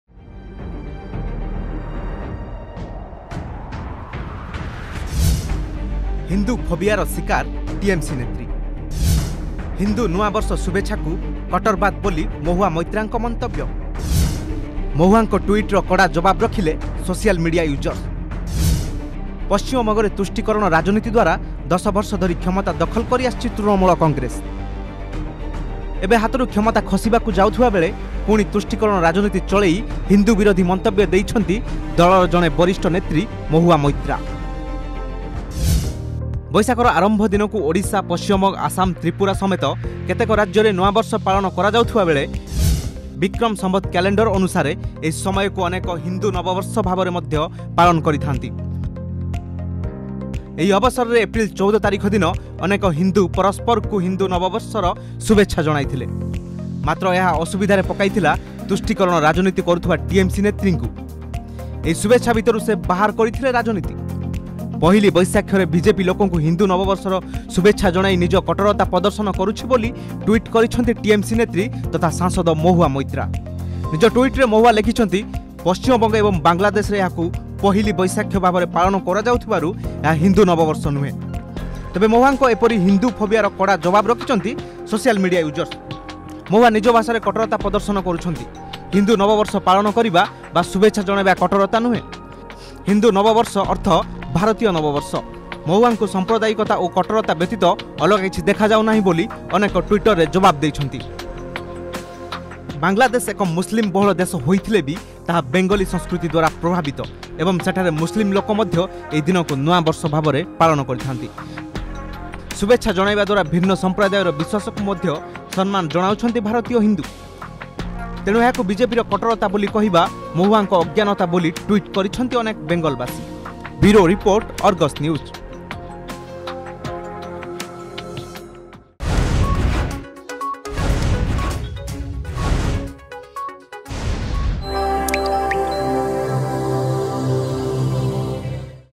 ‘ହିନ୍ଦୁ ଫୋବିଆ’ ର ଶୀକାର ଟିଏମ୍‌ସି ନେତ୍ରୀ
ହିନ୍ଦୁ ନୂଆ ବର୍ଷ ଶୁଭେଛାକୁ କଟ୍ଟରବାଦ ବୋଲି ମହୁଆ ମୋଇତ୍ରାଙ୍କ ମନ୍ତବ୍ୟ। ମହୁଆଙ୍କ ଟ୍ୱିଟ୍‌ର କଡ଼ା ଜବାବ୍ ରଖିଲେ ସୋସିଆଲ୍ ମିଡ଼ିଆ ୟୂଜର୍ସ...